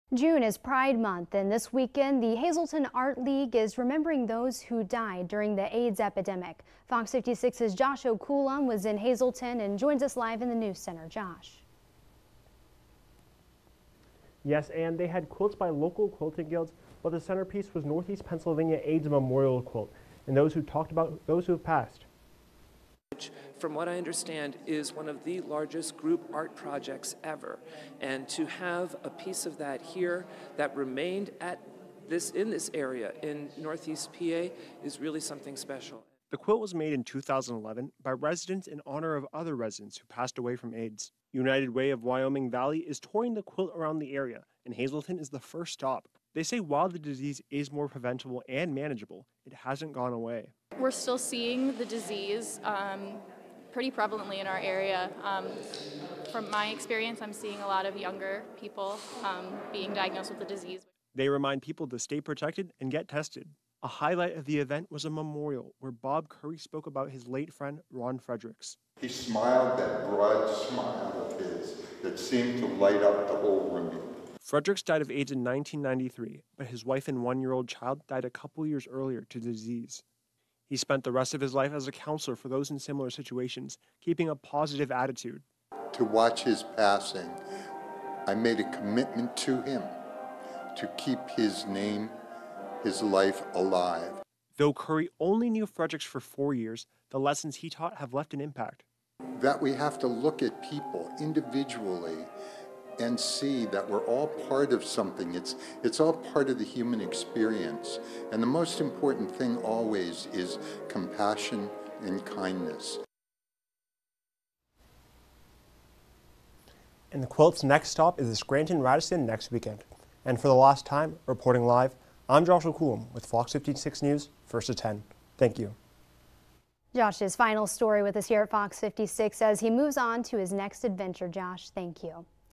Hazleton Art League honors AIDS victims
June is Pride Month and the Hazleton Art League is remembering all those who passed during the AIDS Pandemic this weekend.

They art league had quilts by local quilting guilds but the centerpiece was Northeast Pennsylvania’s AIDs Memorial Quilt and talked about those who have passed.